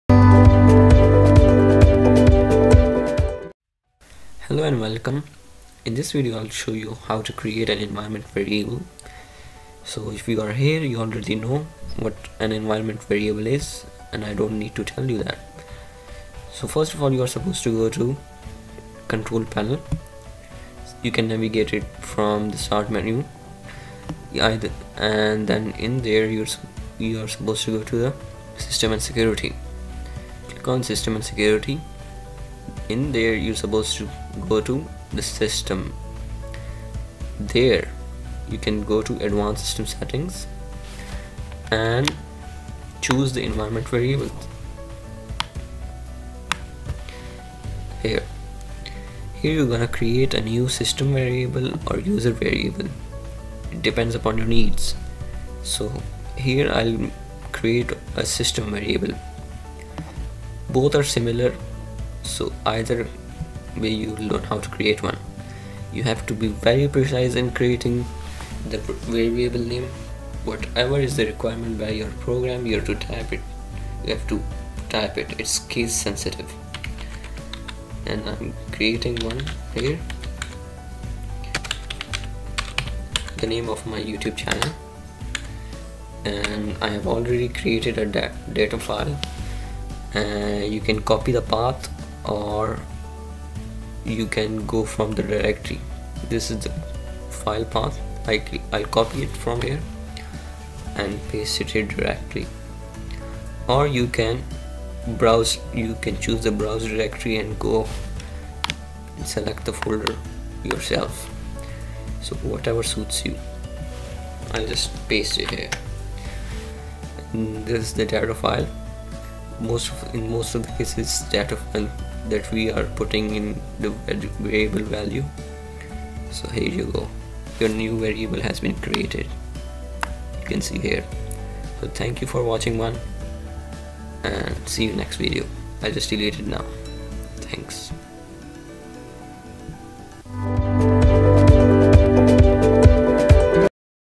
Creating Environment Variables Environment Variable (system/ user) | Adding path values on windows
The Versatile Engineer presentation  This video will help you in creating a new user or system variable and assigning path value to it. Happy Learning!